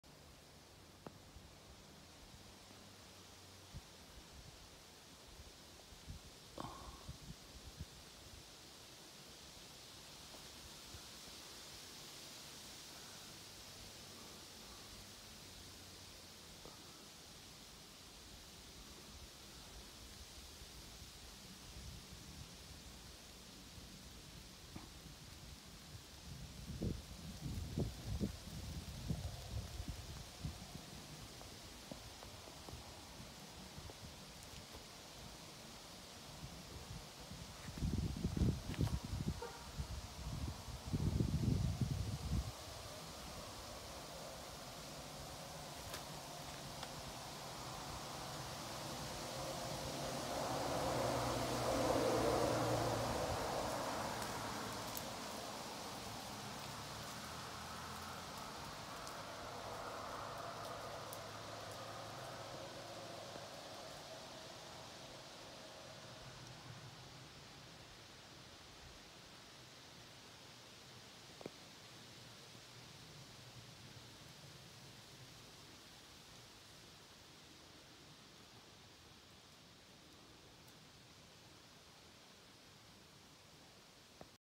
Aspen trees in Fall
Aspen trees looking gorgeous in Fall on Pikes Peak Highway in Colorado. Listen to how the wind on the Aspens sounds like rain.